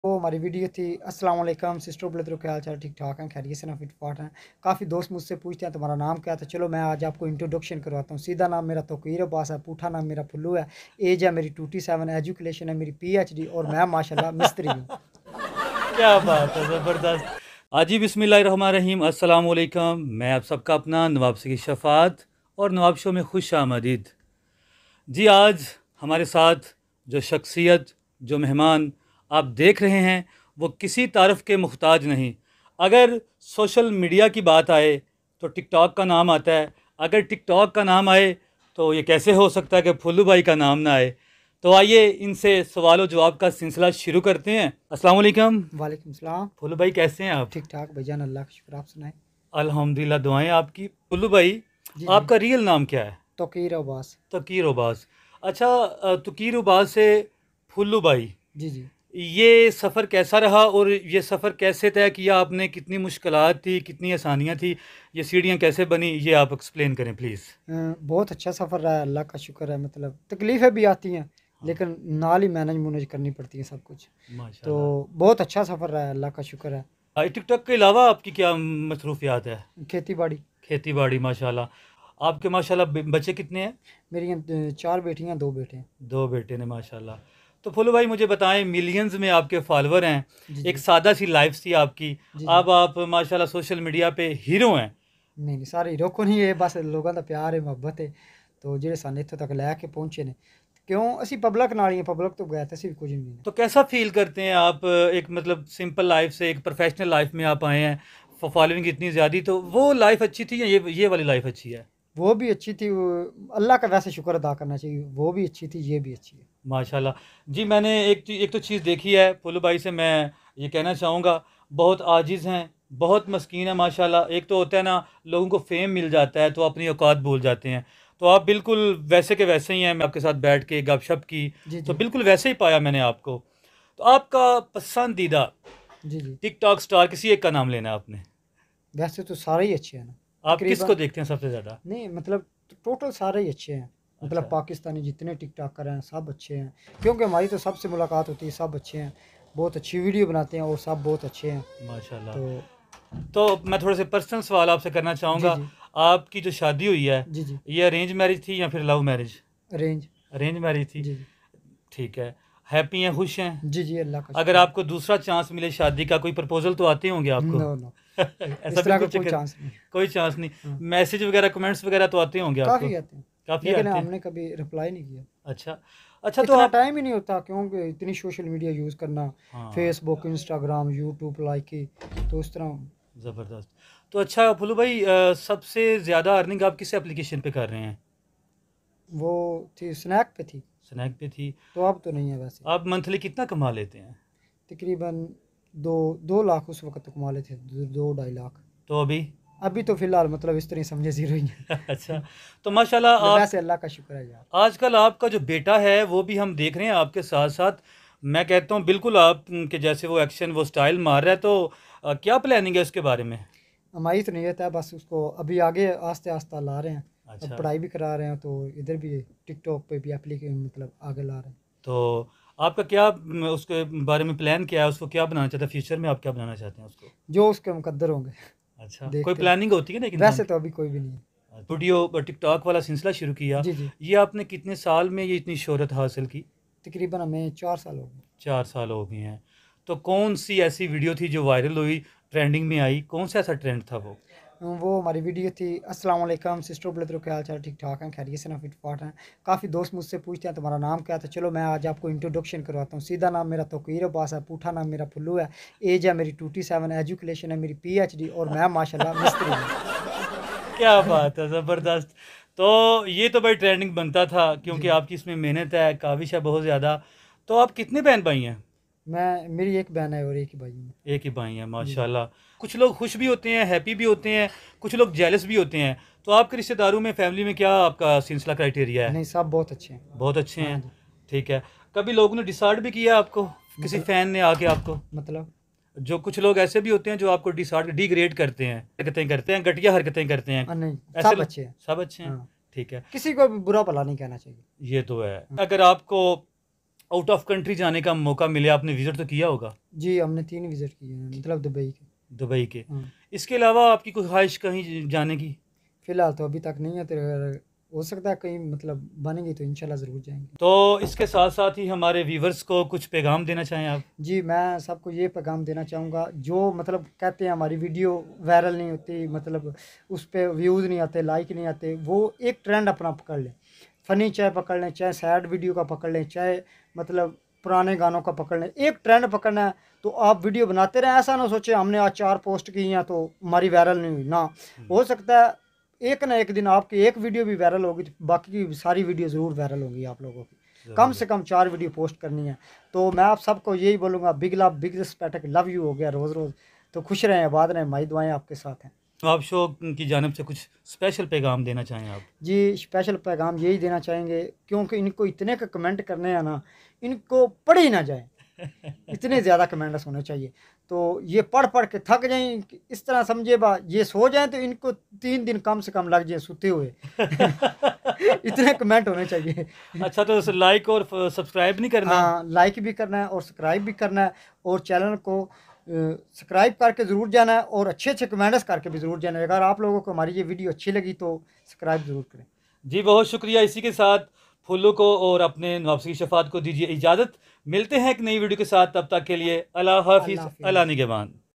Phullo Most interesting interview | Nawab Show
Tiktoker Phullo Aj Kal Kahan Hai ? | Nawab Show

Latest interview of a pakistani famous tiktok star PHULLO with Nawab Syed Shaffat
A very funny interview from sialkot
i hope you all will enjoye this interview and give your feedback in the comment,

                                  PEACE ✌